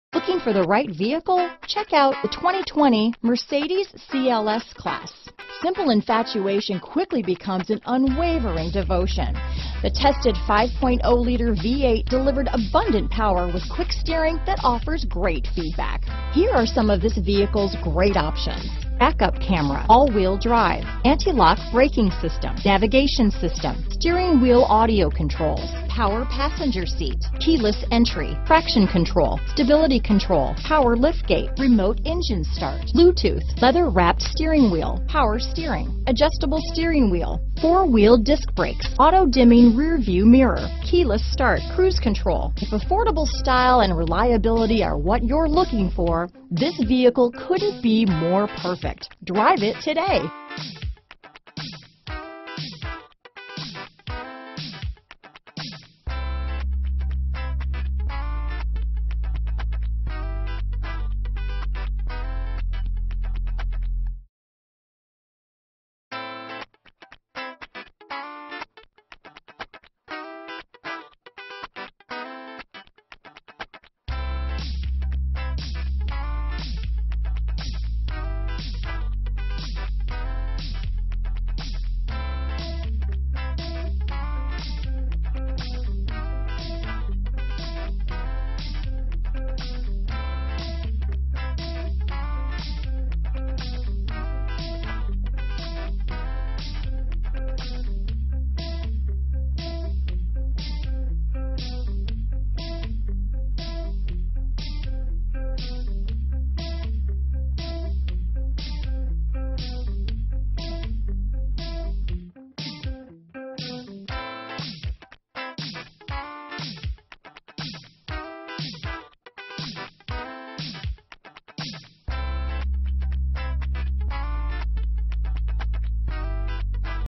2020 Mercedes-Benz CLS available in Dublin, Columbus, Delaware OH LA050485
New 2020 Mercedes-Benz CLS available in Dublin, Ohio at Crown Eurocars Dublin. Servicing the Columbus, Delaware, Powell, Westerville, OH area.
2020 Mercedes-Benz CLS CLS 450 - Stock#: LA050485 - VIN#: WDD2J5KB4LA050485